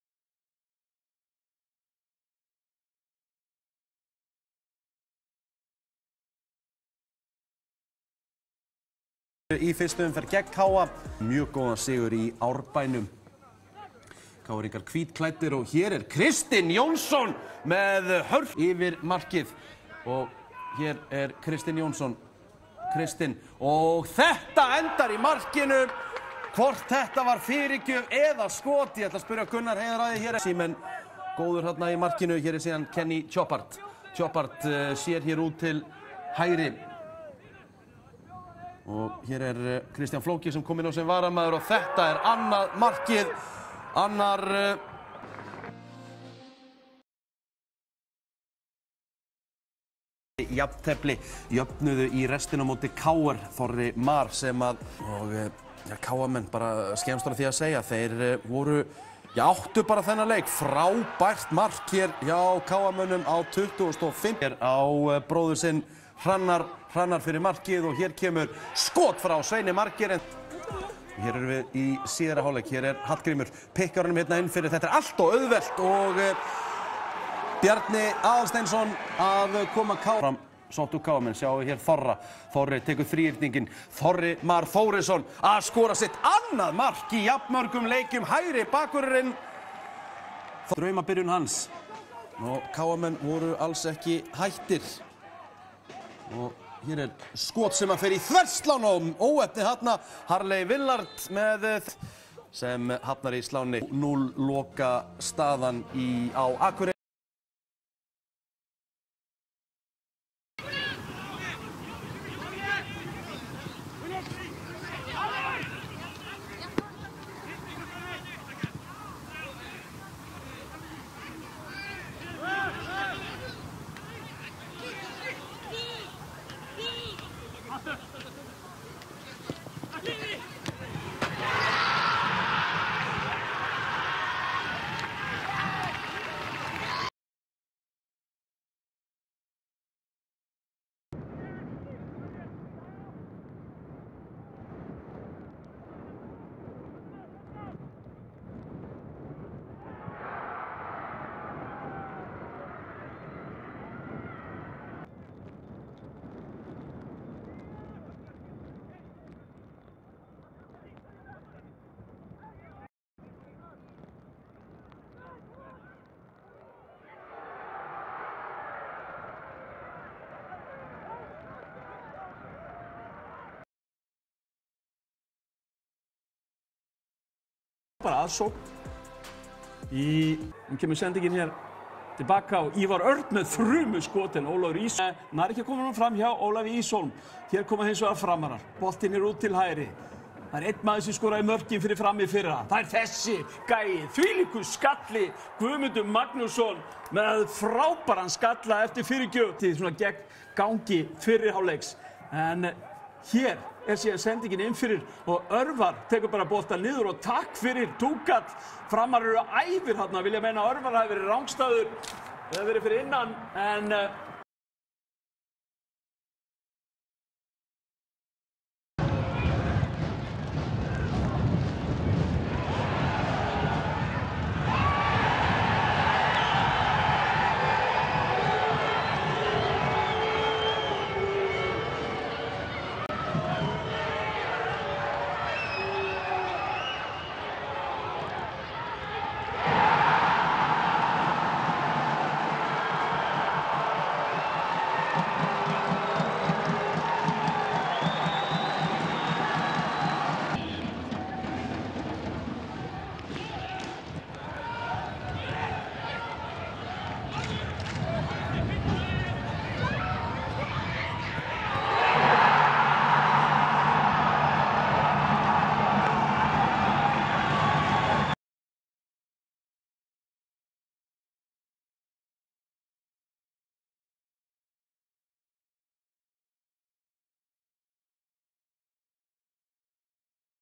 Besta Deildin 2023 (2nd round, men)
Here you can see all goals from 2nd round of Icelandic league.
6 games = 12 goals

Results:
Keflavík - KR Reykjavík 0:2
KA Akureyri - ÍBV 3:0
FH Hafnarfjordur - Stjarnan 1:0
Víkingur - Fylkir 2:0
HK Kopavogur - Fram 1:1
Valur - Breidablík 0:2